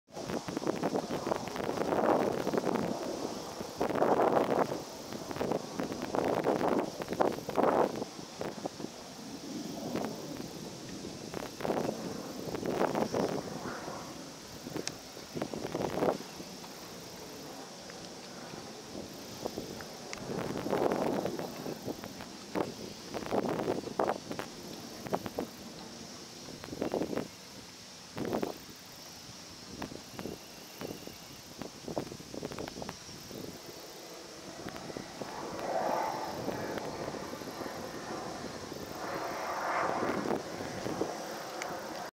小川原湖のオオバン親子
小川原湖のオオバン親子の様子。